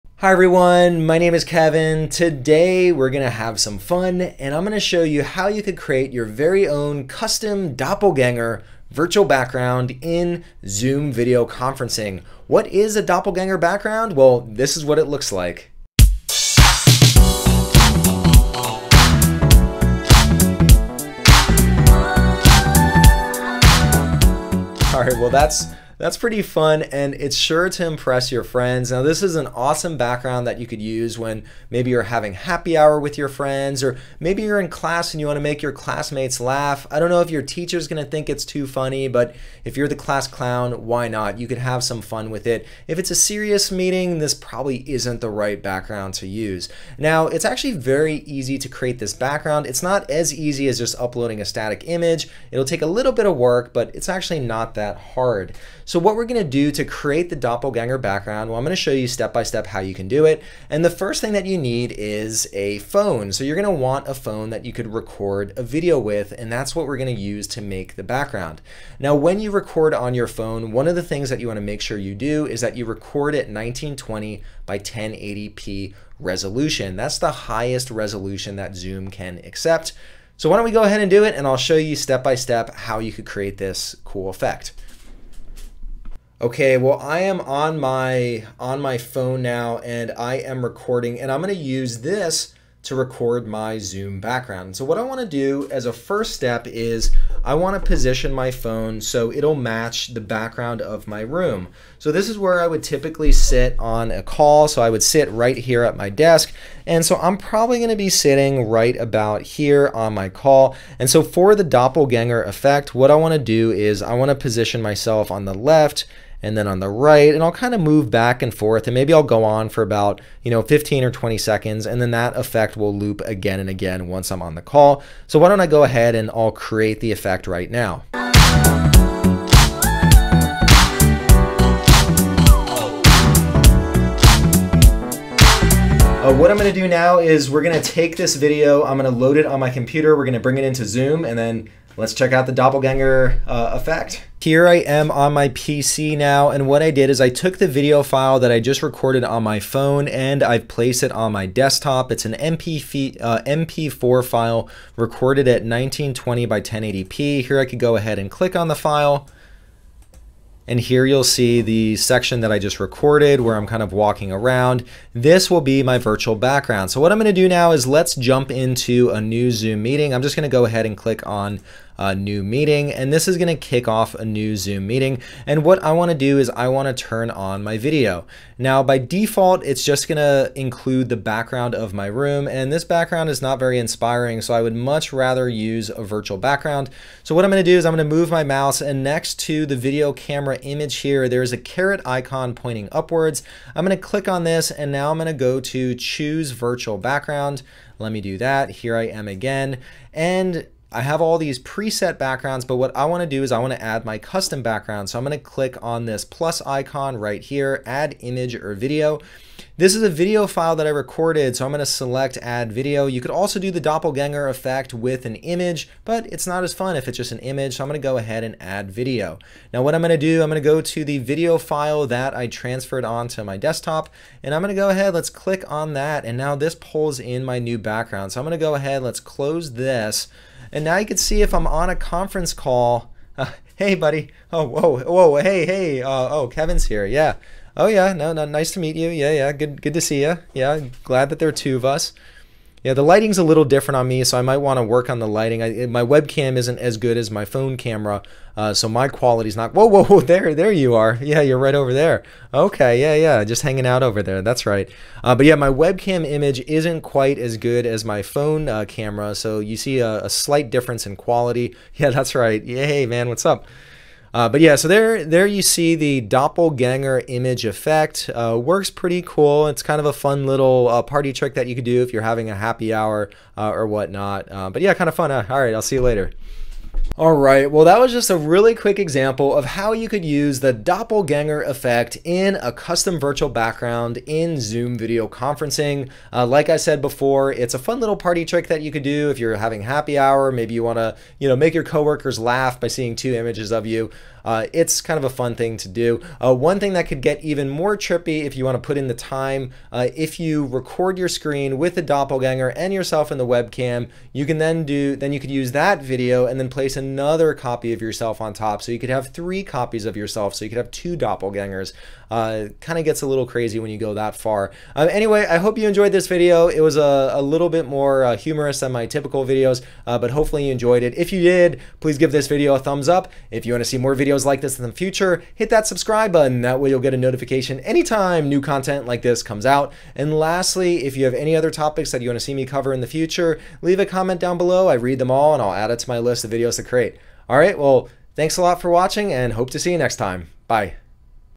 How to make Doppelganger Virtual Background in Zoom
In this step-by-step tutorial video, learn how to create a Doppelganger Virtual Background in Zoom Video Conferencing.